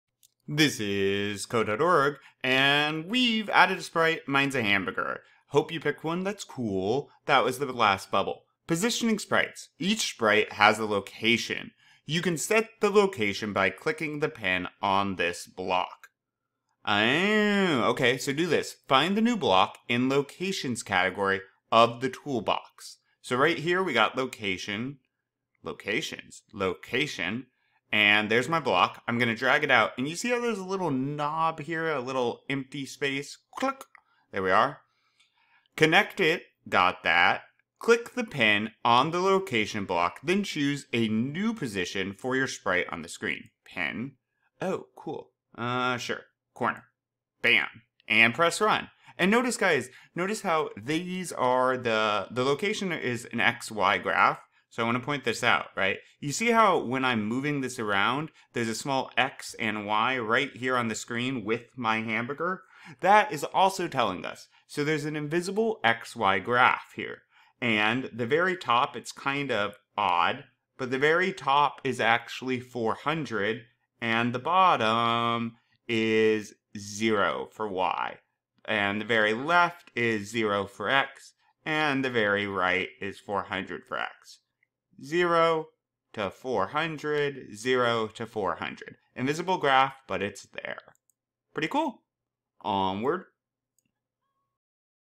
Code.org Express Lesson 6.9 Making Sprites | Tutorial with Answers | Course F Lesson 3.9
Making Sprites is part of Code.org's C.S. Fundamentals course. In Course F it is lesson 2 and in the Express Course, it is lesson 6. This lesson is an excellent way to learn to code. Each part challenges students to accomplish a new task while learning computer science concepts. C.S. Fundamentals is often used in elementary and middle schools. In this video, I go through each bubble/puzzle/part, walking through step-by-step how to get the right answers for this lesson. Let's have fun, learn to program, build cool stuff, and then go change the world!